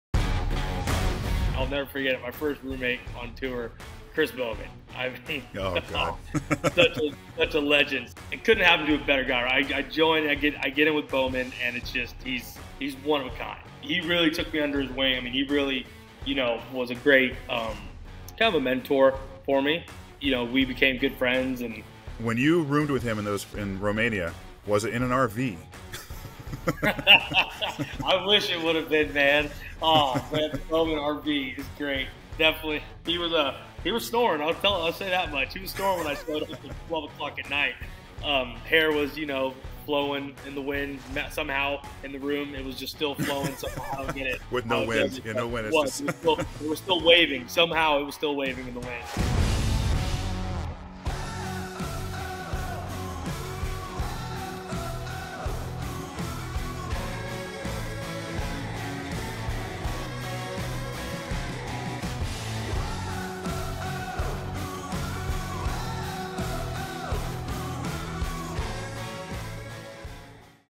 Eagle-Eyed Rugby Podcast - Dino Waldren on rooming with Chris Baumann
🎙 NOLA Gold's Dino Waldren talks about his first roommate on tour, Chris Baumann.  👀 Look for the full Eagle-Eyed Rugby Podcast interview this Wednesday!